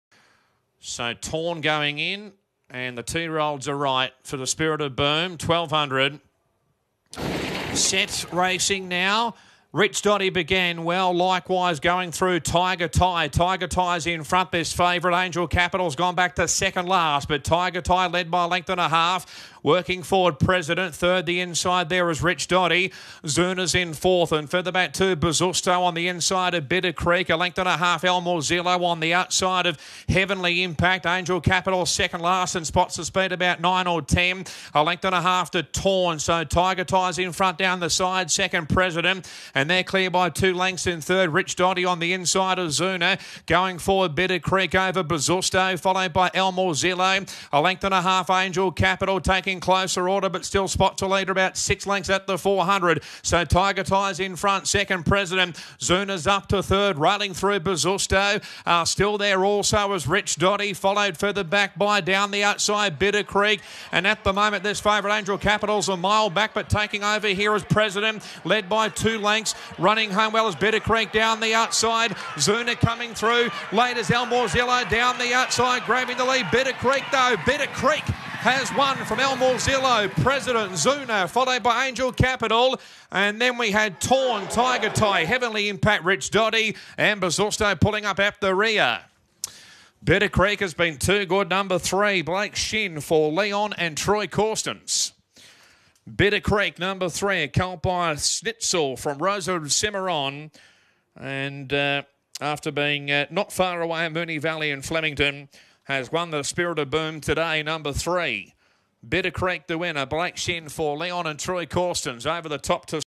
2024 Sprit Of Boom Classic
The Leon & Troy Corstens Trained Bittercreek Wins The Sprit Of Boom Classic At Doomben Racecourse This Afternoon.